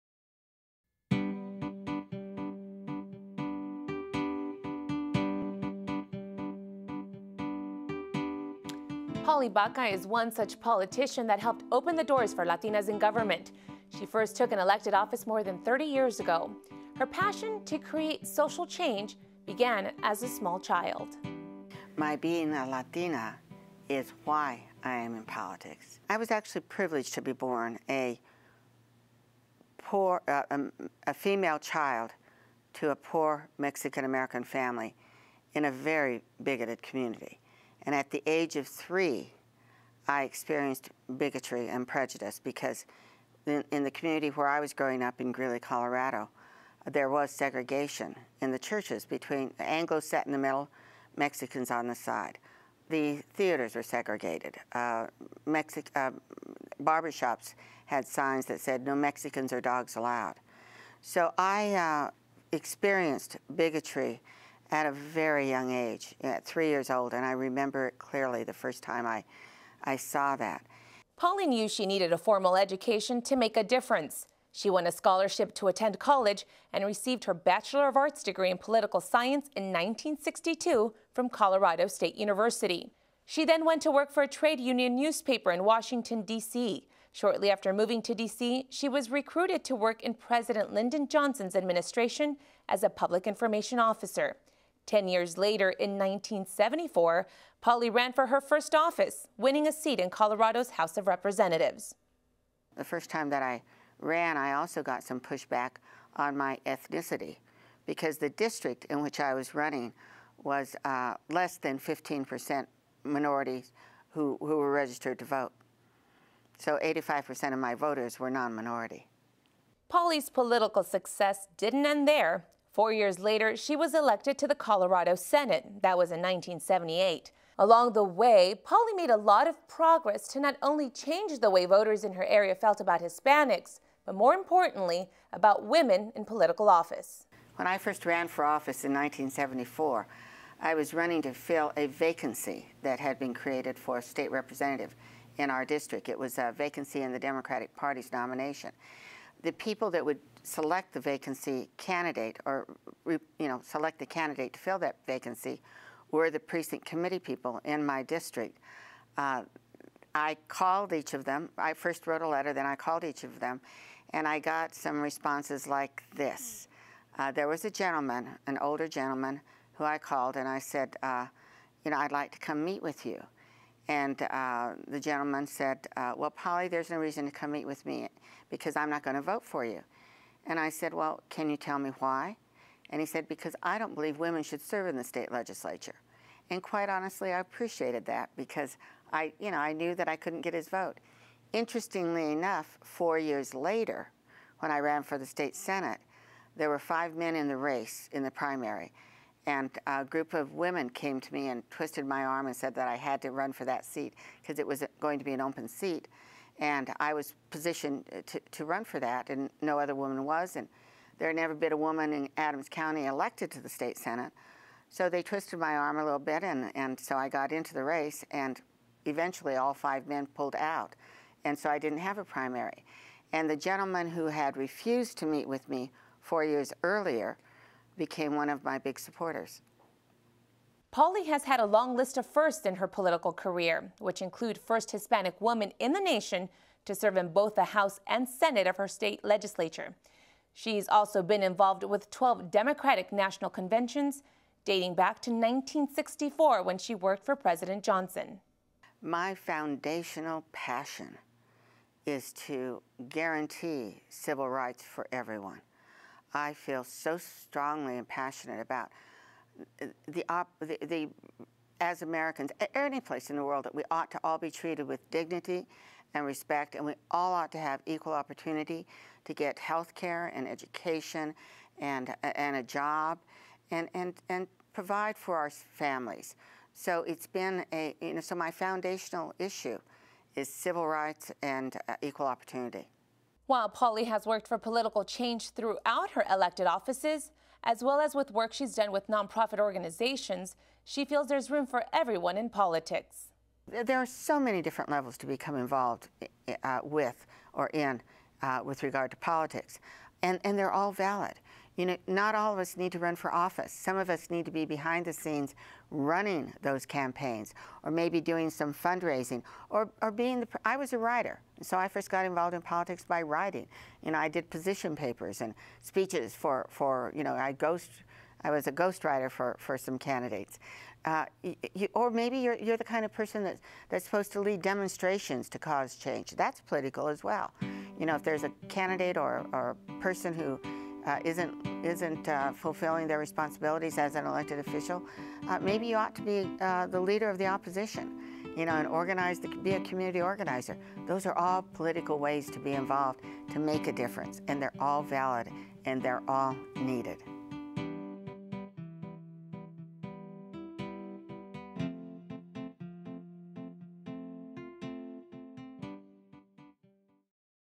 Latina Lens: Polly Baca
(Denver, Colorado) Among her many distinctions, Polly Baca was the first Hispanic woman in the nation to serve in both the House and Senate of her state legislation (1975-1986.)  She later served as Special Assistant to President Bill Clinton and Director of the United States Office of Consumer Affairs in Washington, D.C.  Most recently Ms. Baca served as a delegate at the Democratic National Convention and was recognized for having been part of 12 Democratic National Conventions throughout her career.